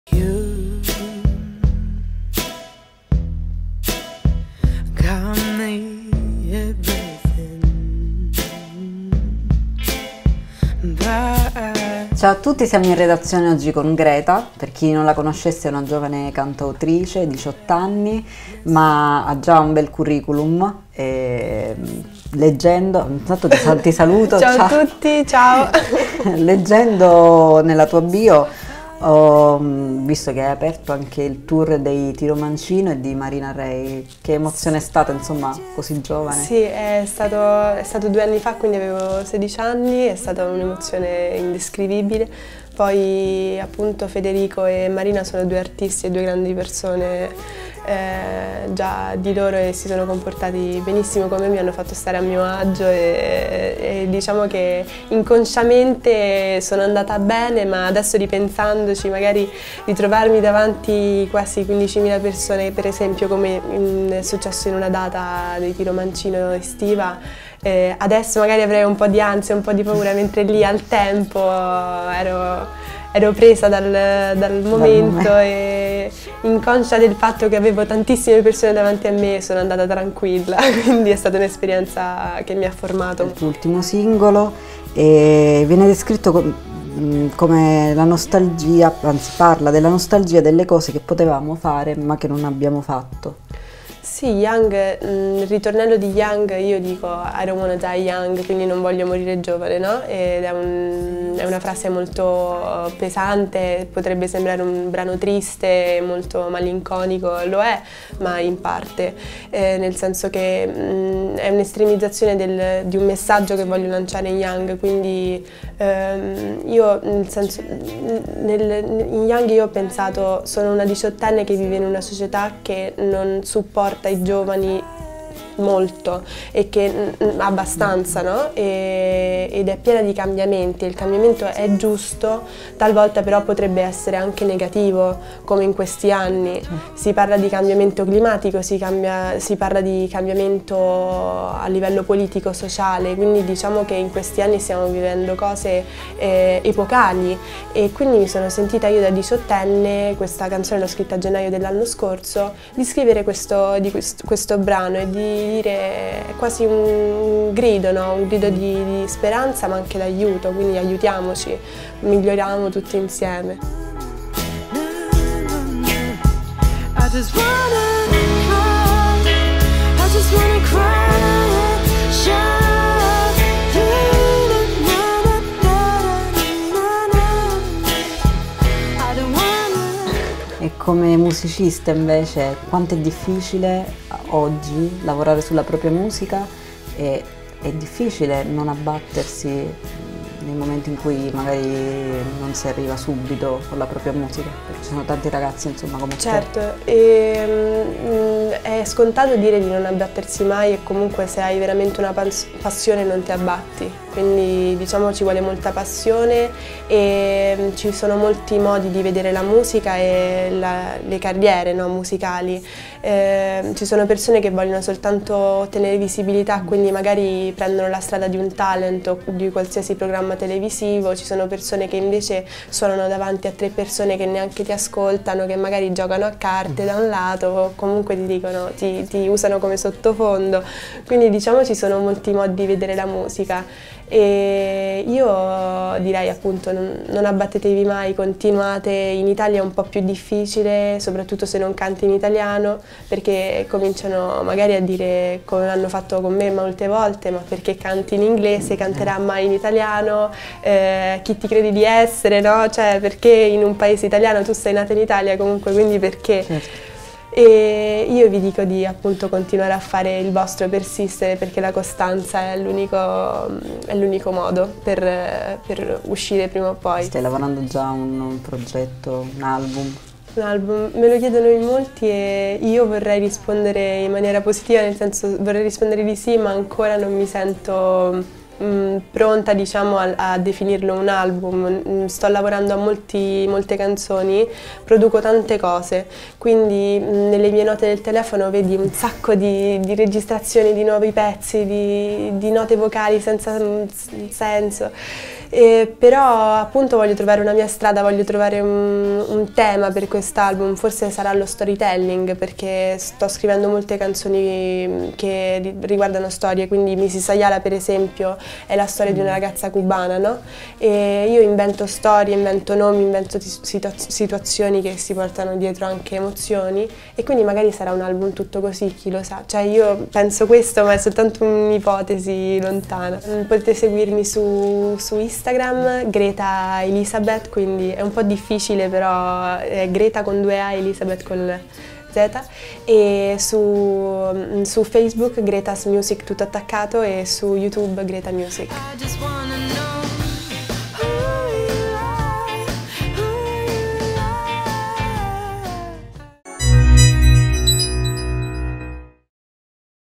Il cantautorato bilingue di Greta: l'intervista
Ha una voce delicata ma forte allo stesso tempo, fa la cantautrice, ha 18 anni e un futuro brillante davanti. È Greta Elisabeth, giovane promessa della musica. L'abbiamo incontrata nella nostra redazione